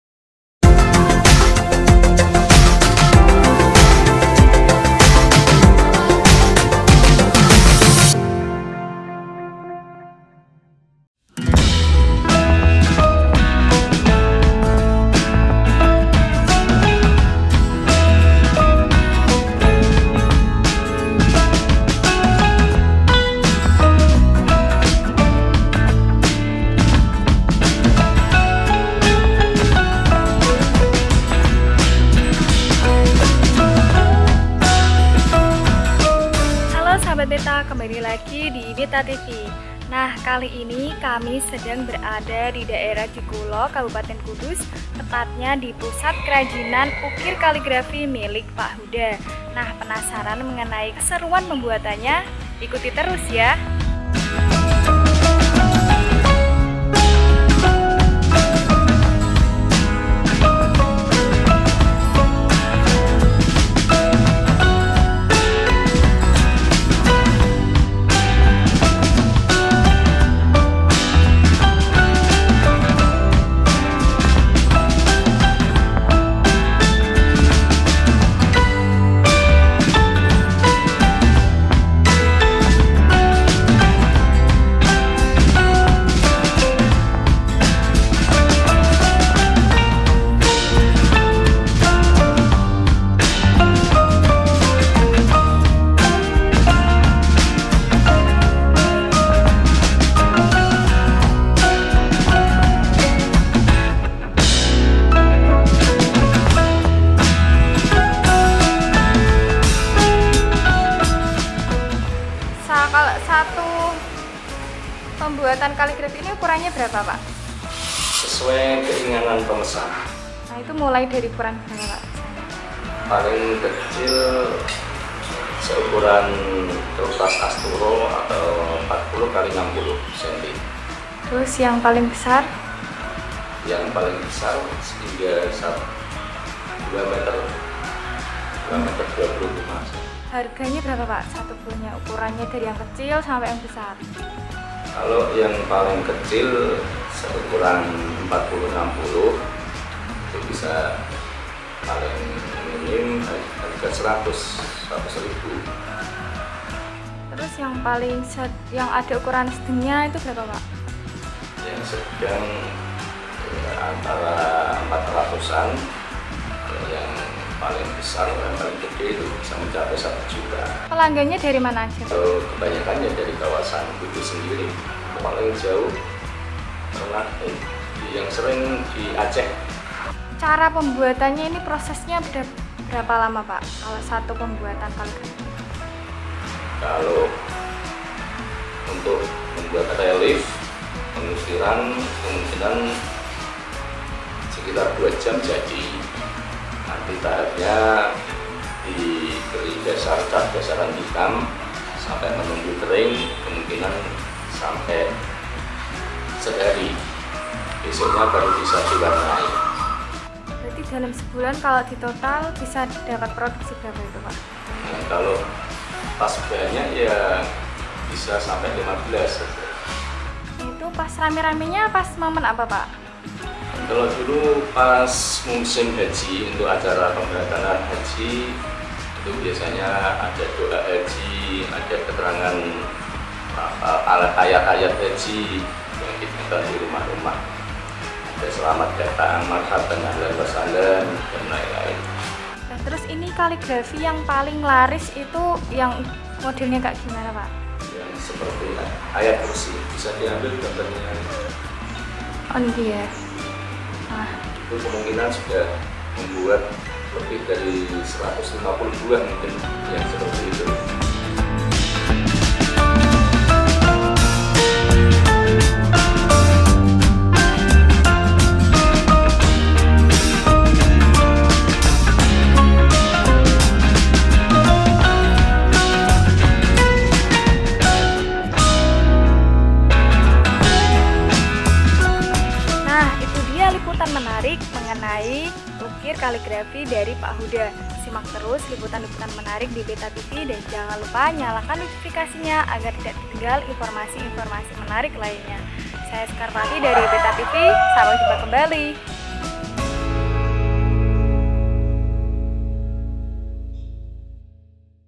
Relief Kaligrafi dari Styrofoam Karya Huda yang Laris Manis
BETANEWS, KUDUS – Relief kaligrafi berbagai model terlihat terpajang di dinding sebuah rumah Desa Jekulo Kidul, Kecamatan Jekulo, Kabupaten Kudus. Karya seni Islami itu merupakan karya Miftahul Huda (44) yang jadi contoh saat ada orang yang ingin memesan kepadanya. Yang menarik, kaligrafi itu rupanya terbuat dari styrofoam yang diukir hingga punya tampilan mempesona.

Pemilik Griyo Kaligrafi itu memang sudah cinta mati dengan kaligrafi sejak masih duduk di bangku sekolah. Keahlian yang terus diasahnya itu kemudian bisa jadi ladang rezeki keluargnya. Saat ini, ia telah menggarap berbagai model ukiran kaligrafi, seperti kalimat Laila Ha Illallah, nama anak, Surat al-Ikhlas, Ayat Kursi, dan Selawat Nariyah.